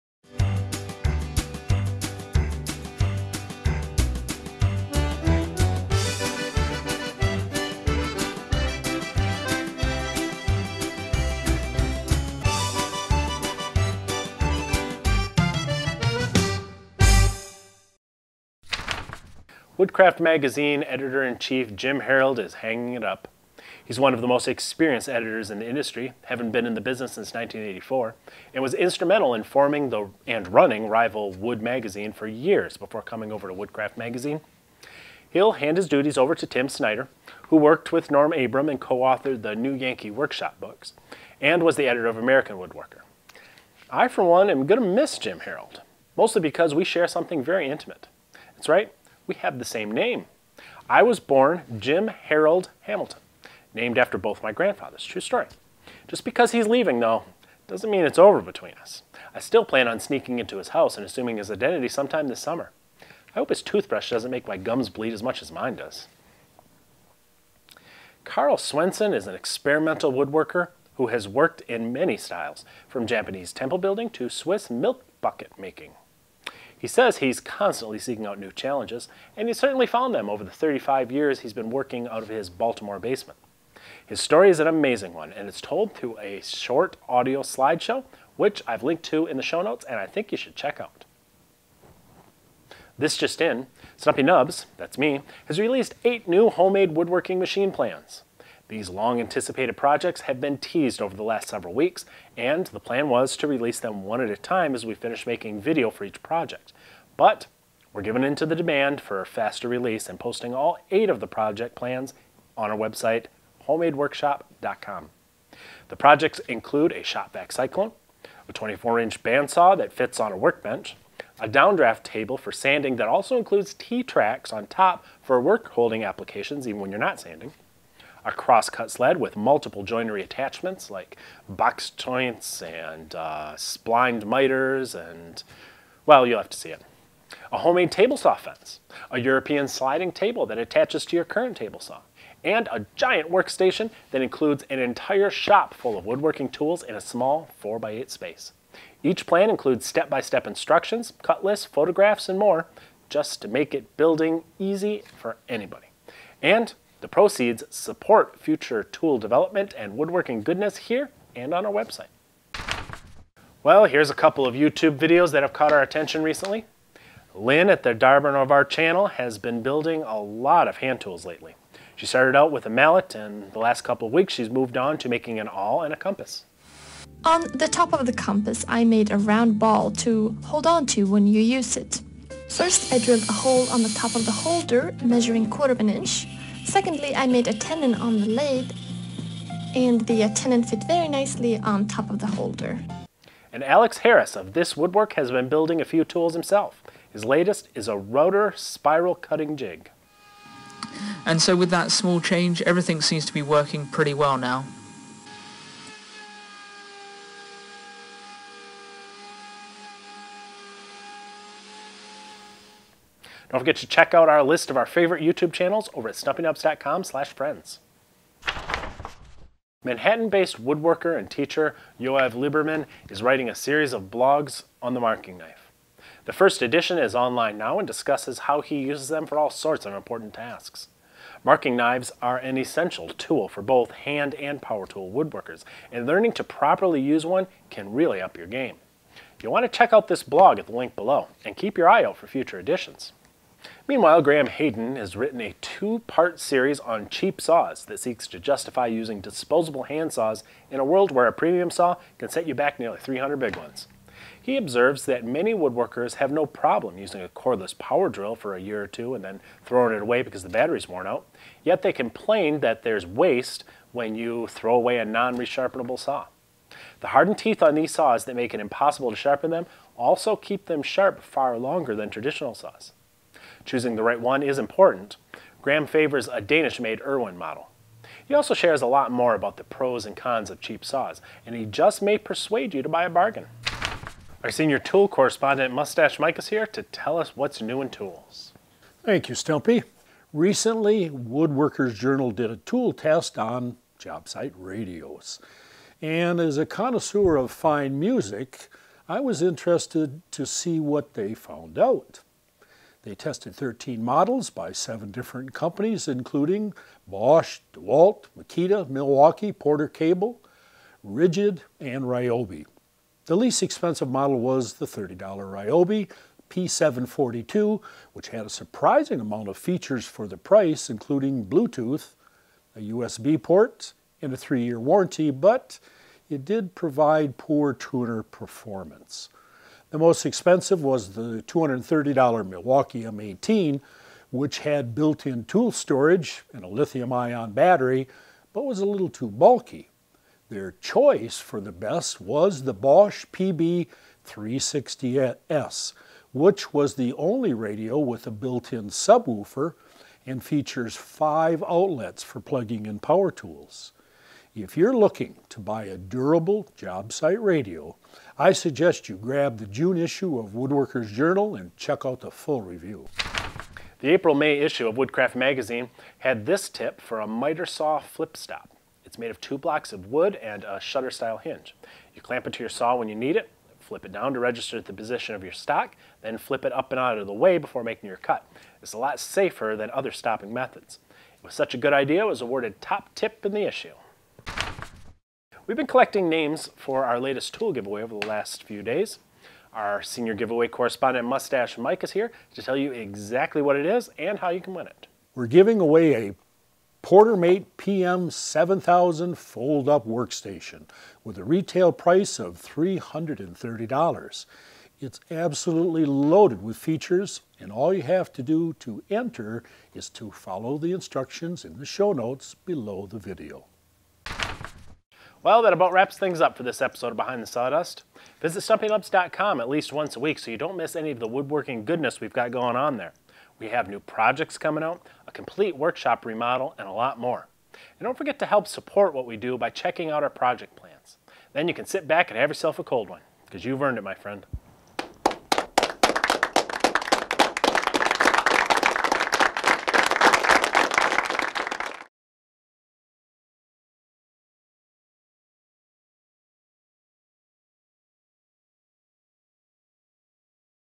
TODAY'S WOODWORKING NEWS #21- Using cheap handsaws, job site radios and more!
How to enter the giveaway: You must be a subscriber to our YouTube channel AND our website newsletter.